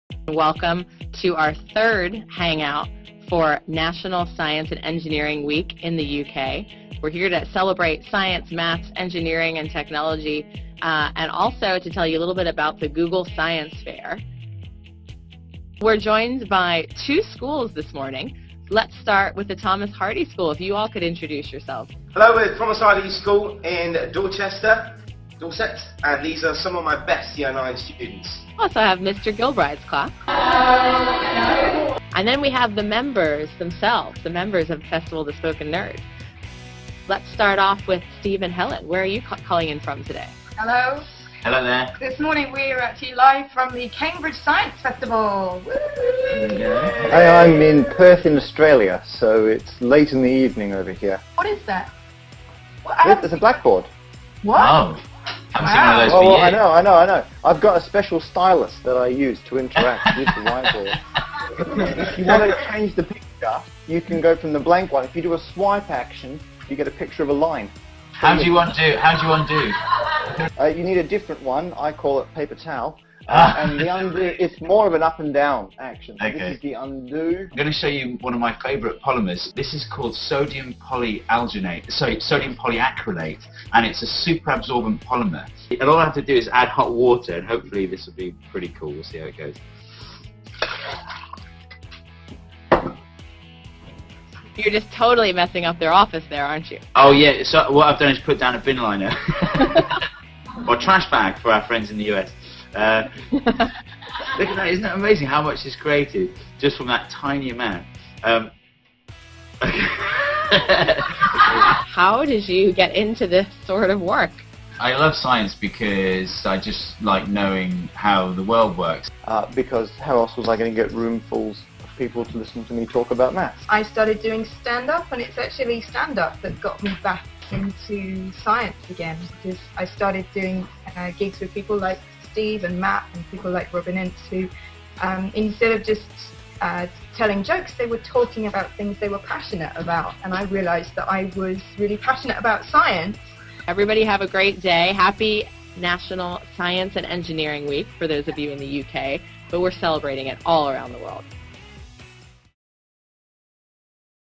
Highlights: Google Science Fair Hangout with the Festival of the Spoken Nerd
In this Google+ Hangout the Google Science Fair team and some schools in the UK get together with the group Festival of the Spoken Nerd (FOTSN). FOTSN is a comedy group made up of 2 scientists and 1 mathematician. In the Hangout they help us take a humorous look at interesting science and maths questions.

This Hangout took place on National Science & Engineering Week in the UK.

To learn more about the Festival of the Spoken Nerd and their upcoming performances,